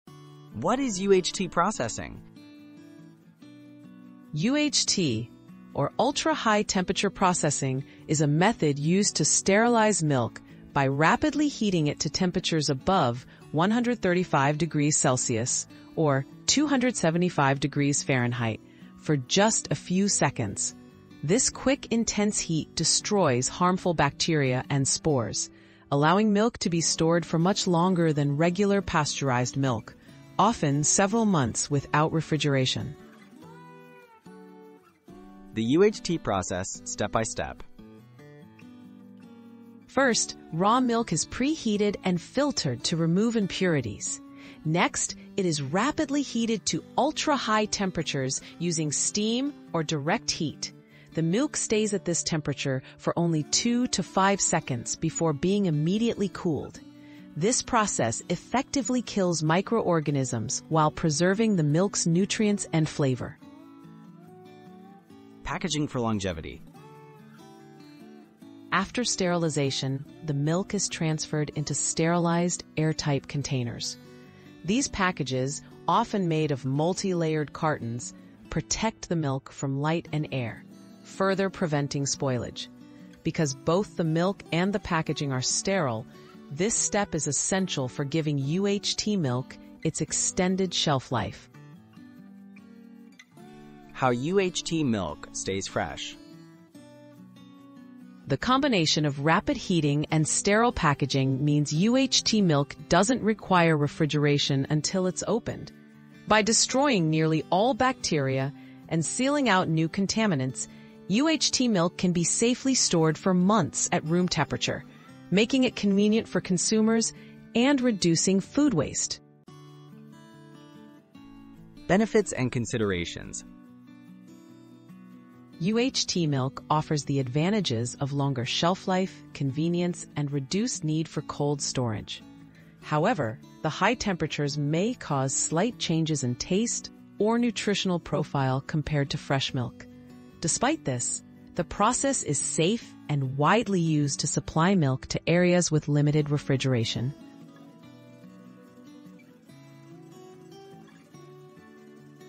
How UHT Processing Extends Milk Shelf Life
UHT Milk 👉 Explore how ultra-high temperature (UHT) processing sterilizes milk to extend its shelf life without refrigeration—preserving nutrients and flavor in a convenient, long-lasting product.

00:00 🔹 What Is UHT Processing?
00:35 🔹 The UHT Process Step by Step
01:05 🔹 Packaging for Longevity
01:33 🔹 How UHT Milk Stays Fresh
02:01 🔹 Benefits and Considerations

📌 Food Science and Nutrition Technology 🔸 Food Processing Technologies 🔸 How UHT Processing Brings You Long-Life Milk